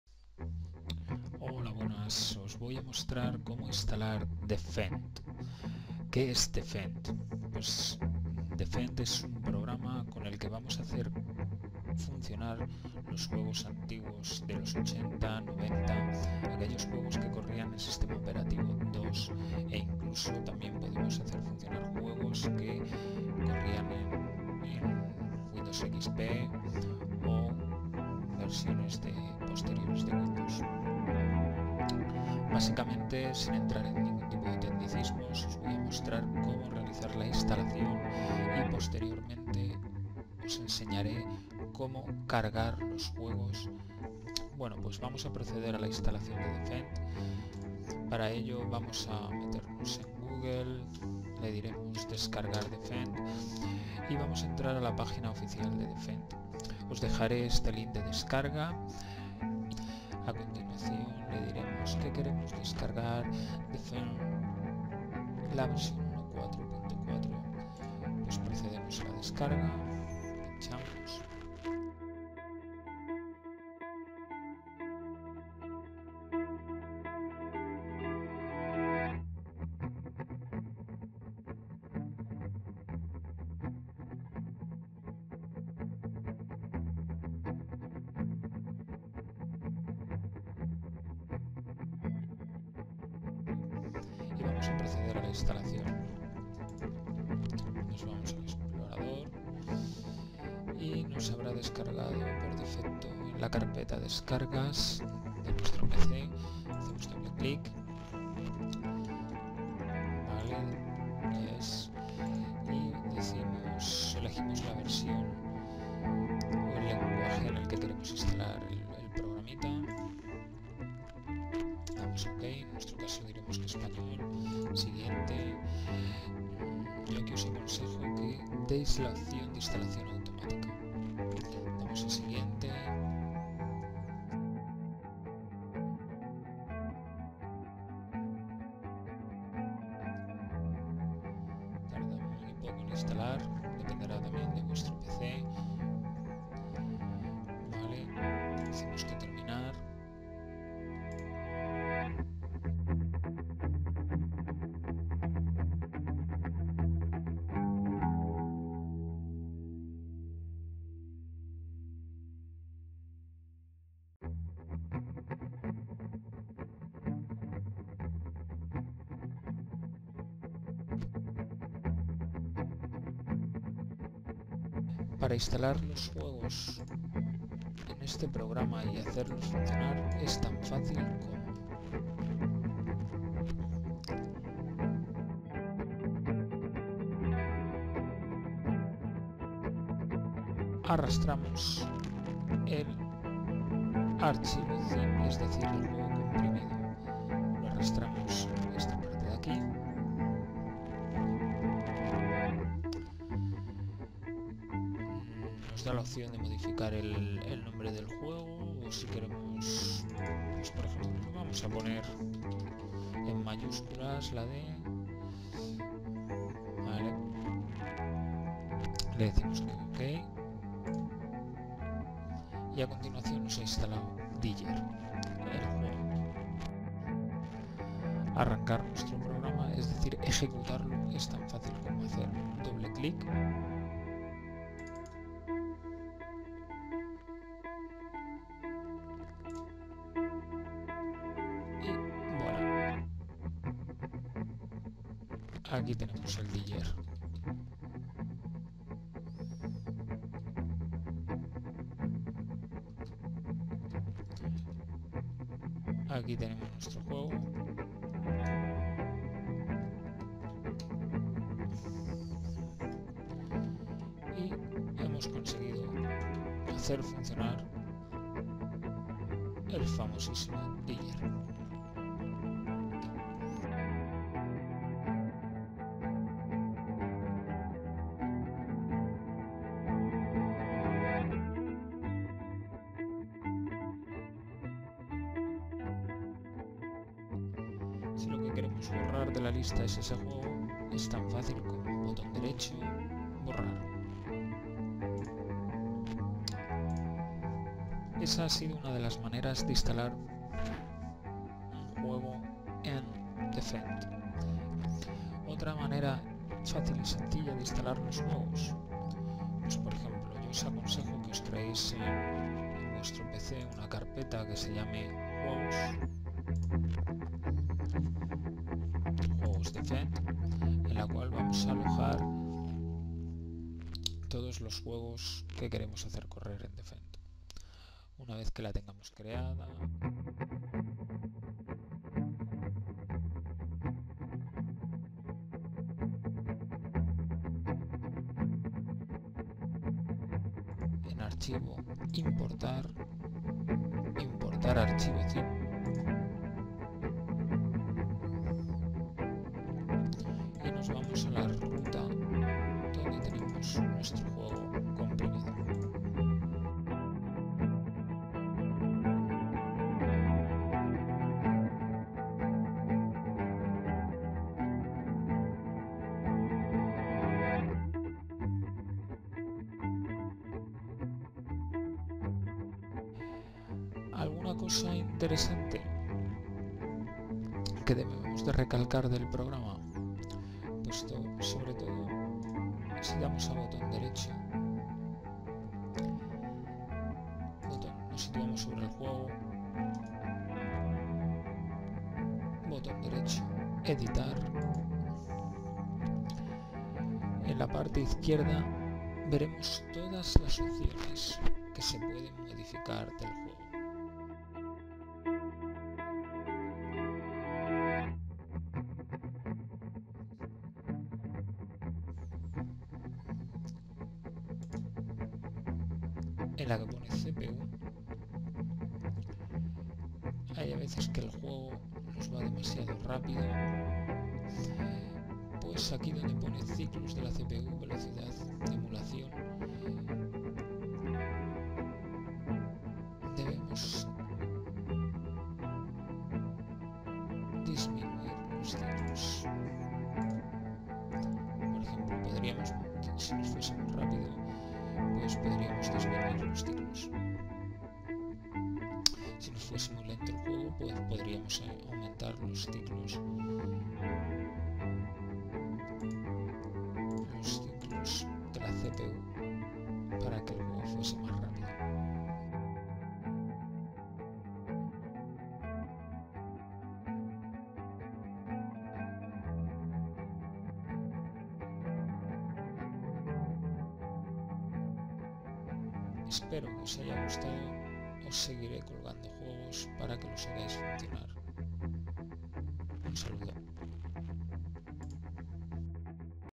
Como jugar a juegos antiguos de PC con D-fend
En este video os mostraré como instalar D-fend y  hacer funcionar juegos antiguos de los 80, 90 con el .

Próximamente Os subiré mas vídeos sobre la configuración y las muchas opciones que podemos tener con D-fend .